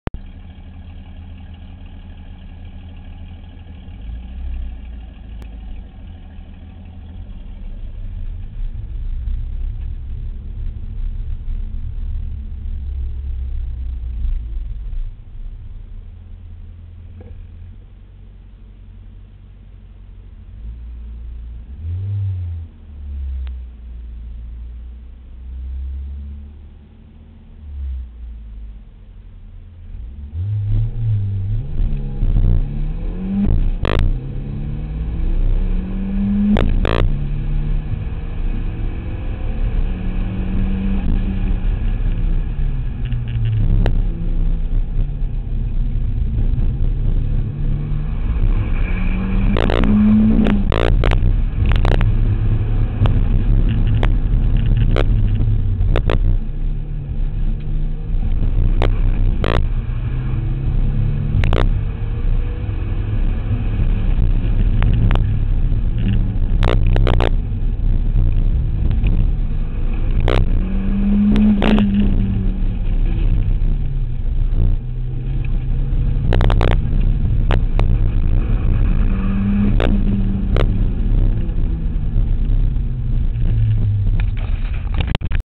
Pittsworth 2009 RUN 8 - "Carlos #9"
Pittsworth Sprints - 5/6 September 2009
Toyota GT4 - final run for the weekend - not the quickest run however I got to drive it home :)